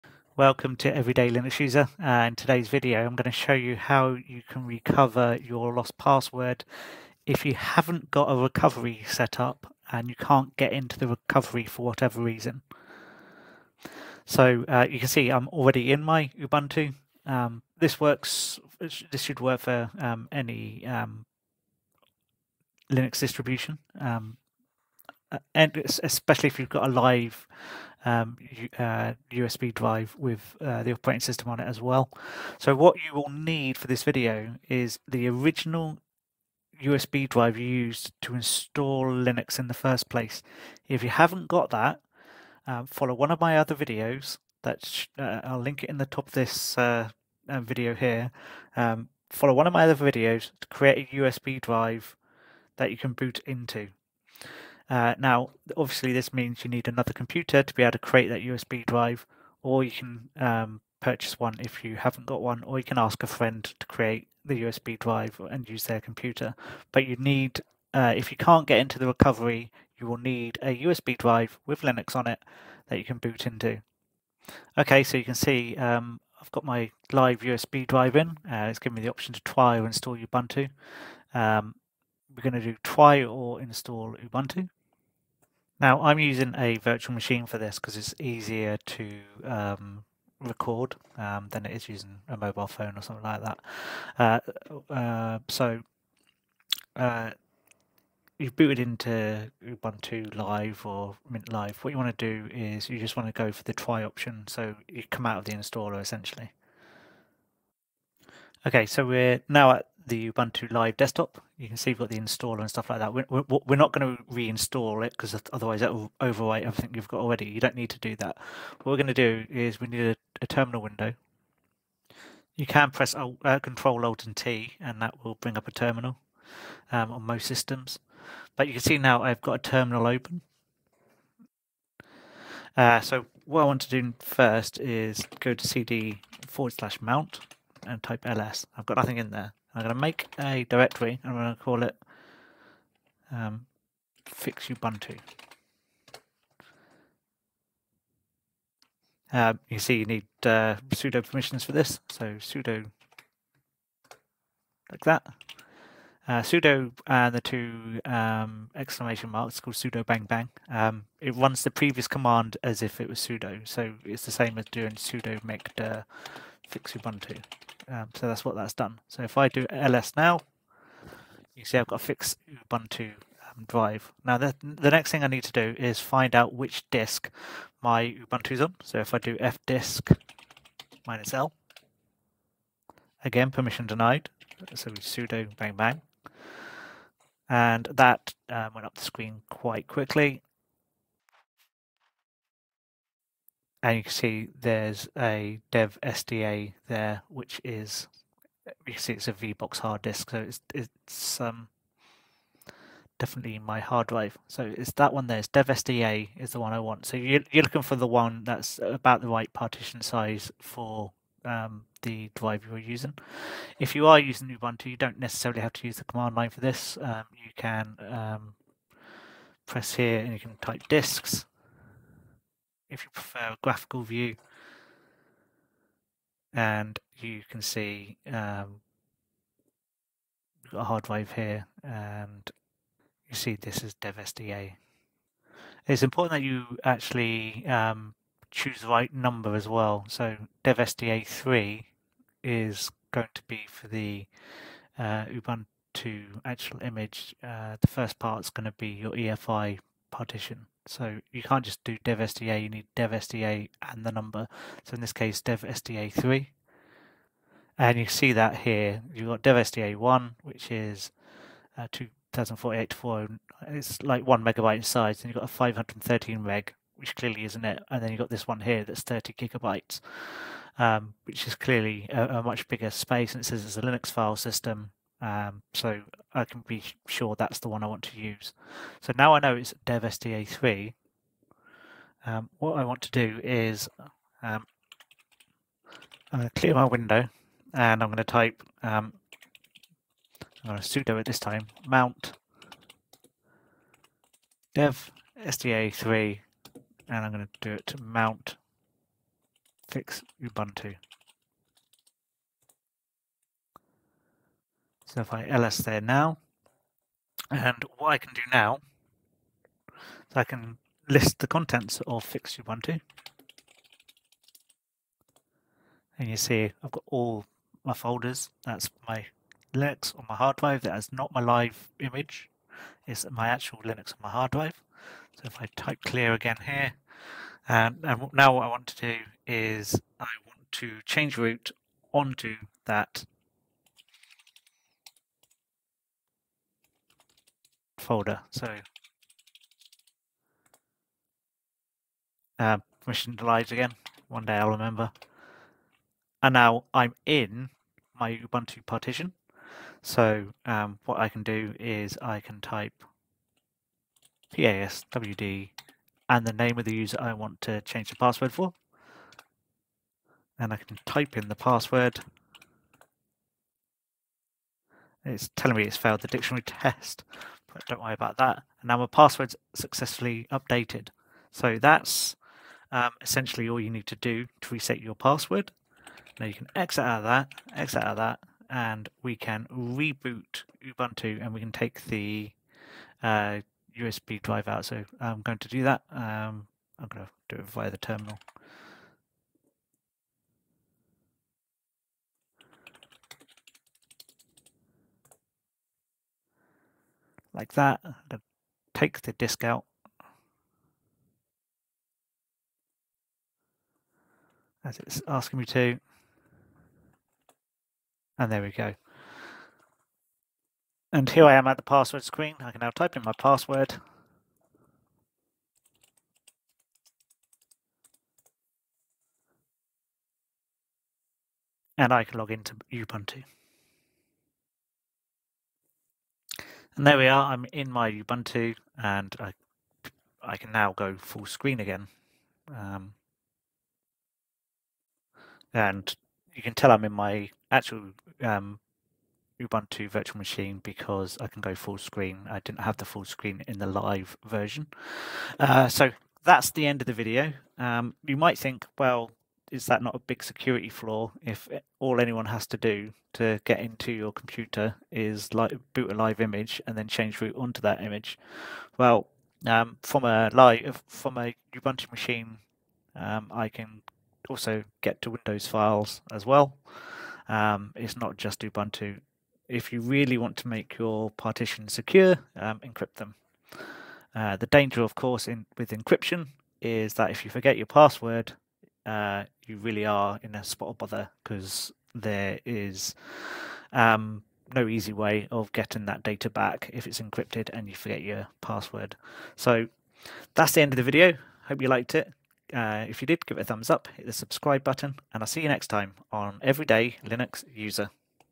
How To Reset A Linux Password Using A Linux Linux USB
This video shows you how to reset a lost Linux password using a Live USB. This will work on all unencrypted drives.